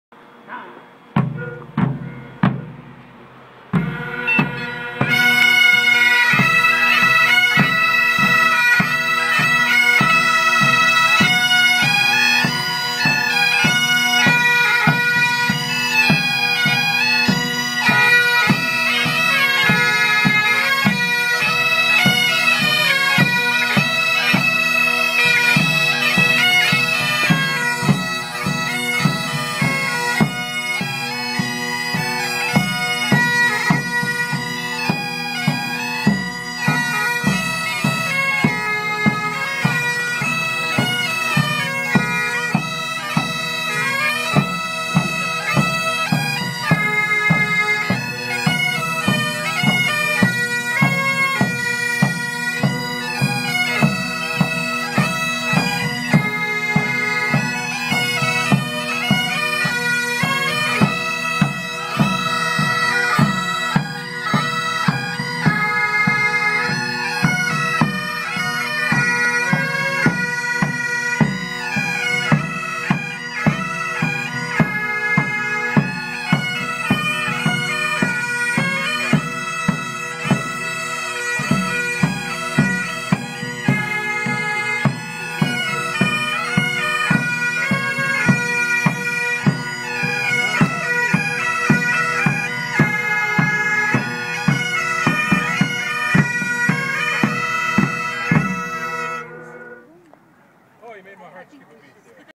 Celtic Pipes and Drums- Waimea 2/4/11
Playing to the sunset over Waimea, Kauai